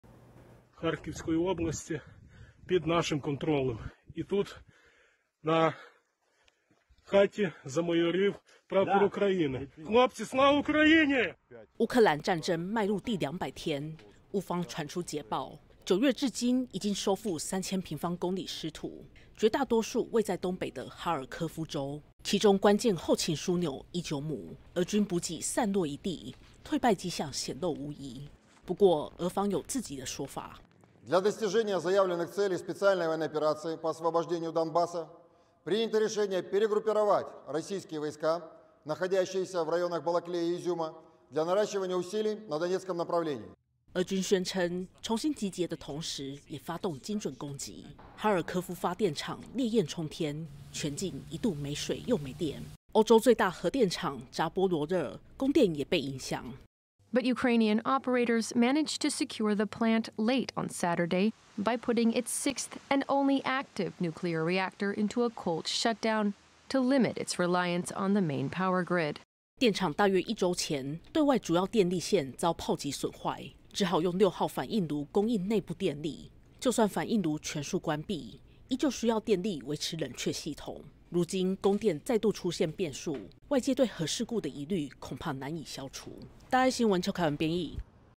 俄烏戰爭200天！收復烏東失土
烏克蘭戰爭在11號邁入第200天，9月以來烏軍大規模反攻，在東北部哈爾科夫地區大有斬獲；俄羅斯則是瞄準電廠等基礎設施，造成大範圍供電中斷，這也威脅到札波羅熱核電廠。這座電廠是歐洲最大核電廠，戰前供應烏克蘭20%的電力，不久前全面中止運轉，不過還是需要電力讓反應爐冷卻，否則只能用柴油發電機來維持冷卻系統，而目前的柴油存量只有10天。

烏克蘭士兵：「哈爾科夫這個村莊，現在在我們控制下，這棟房子上飄揚的是烏克蘭國旗，榮耀歸於烏克蘭。」

烏克蘭戰爭邁入第200天，烏方傳出捷報，9月至今已經收復3000平方公里失土，絕大多數位在東北的哈爾科夫州。其中關鍵後勤樞紐伊久姆，俄軍補給散落一地，敗退跡象顯露無遺，不過俄方有自己的說法。

俄國國防部發言人 科納申科夫：「為了實現解放頓巴斯既定目標，我們決定駐紮在巴拉基亞，伊久姆的俄羅斯軍隊重新集結，加強頓內茨克方向軍力。」

俄軍宣稱，重新集結的同時也發動精準攻擊，哈爾科夫發電廠烈焰沖天，全境一度沒水又沒電。歐洲最大核電廠扎波羅熱，供電也被影響。

聲音 記者：「烏克蘭營運方10號深夜關閉第6號，也是最後一個運作的反應爐，降低對主電力線依賴。」

電廠大約一周前，對外主要電力線遭砲擊損壞，只好用6號反應爐供應內部電力。就算反應爐全數關閉，依舊需要電力維持冷卻系統，如今供電再度出現變數，外界對核事故的疑慮，恐怕難以消除。

RTV APTN：邱楷雯 編譯 周子歆 剪輯